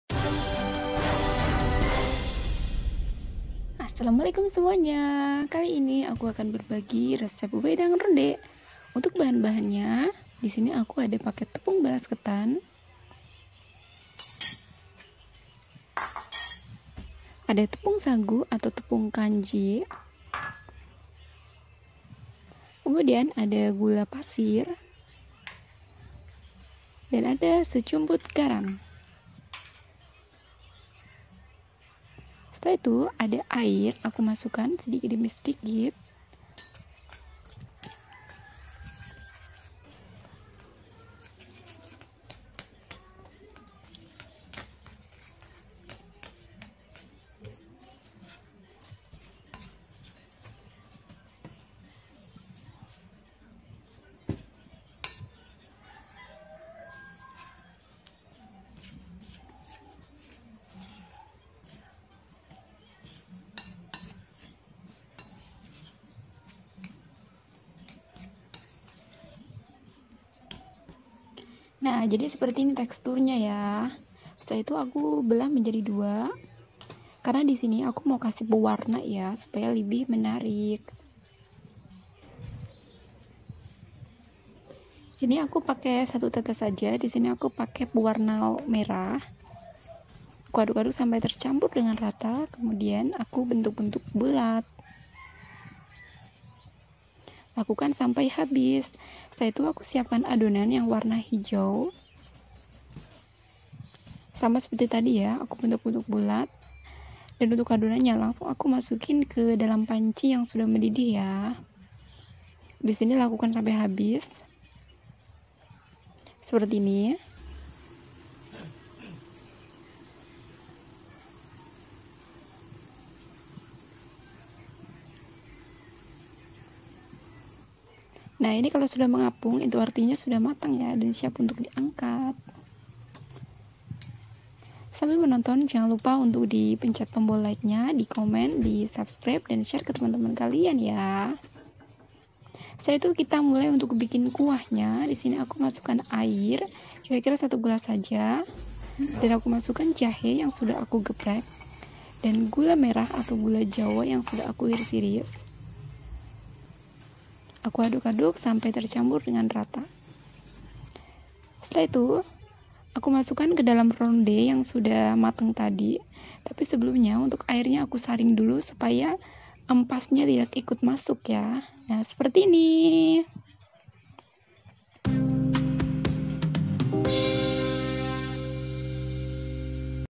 PUNYA JAHE DAN TEPUNG KETAN DI BIKIN MINUMAN INI AJA - RESEP WEDANG RONDE
Assalamu'alaikum semuanya..
Kali ini Aku akan berbagi resep WEDANG RONDE spesial 2020 yang sederhana sekali dan mudah di praktekkan.

Bahan-bahan :

Bahan 1 :
 Tepung beras ketan 200 gr /15 sdm
Tepung kanji atau sagu 30 gr
Gula pasir 1 sdm
Sejumput garam
Air secukupnya

Bahan 2:
Jahe secukupnya
Gula Merah secukupnya
Air secukupnya

Boleh di tambah daun pandan jika suka